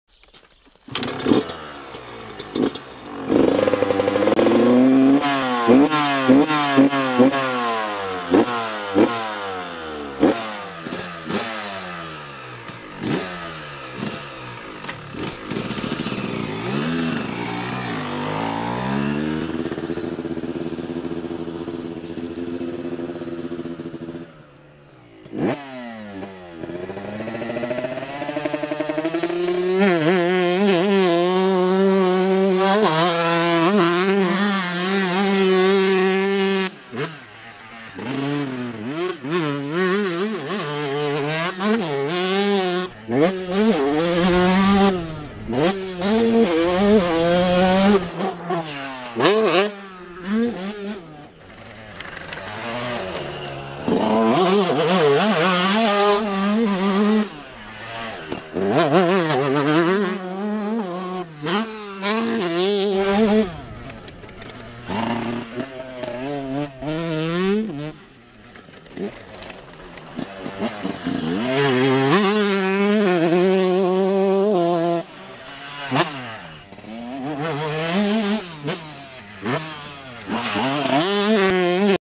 History of the Kawasaki KX 500
It all started with Kawasaki KX450 2-stroke, which was produced from 1974 to 1976. Kawasaki then switched to KX420, which was produced from 1980 to 1982. In 1983 they introduced the legendary KX500, which was a global success. This bike was in production till 2004.